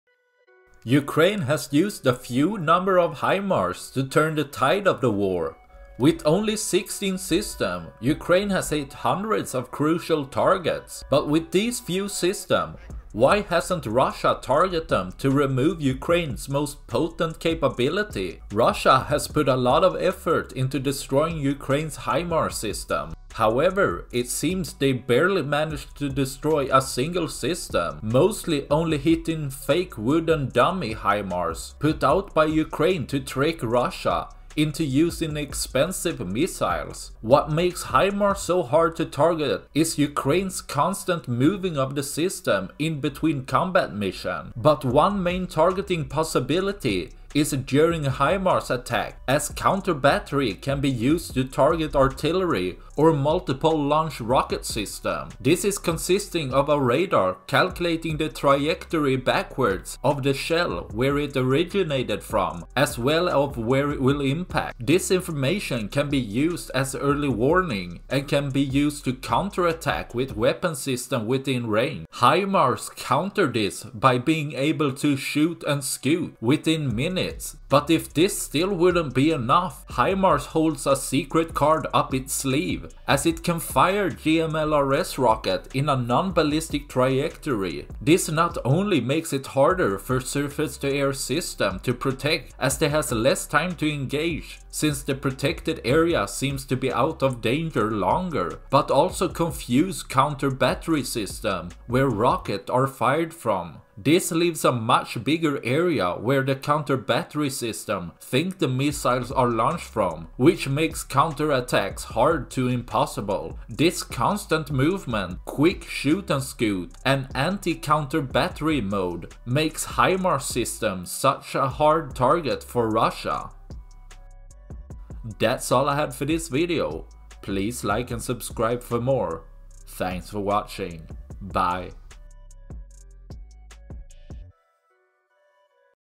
How HIMARS Trick Counter-Battery Radars
Ukraine has only 16 HIMARS, yet Russia seems to have a hard time destroying any of these few system. While it seems like it would be easy, there are a couple of reasons why Russia has such a hard time.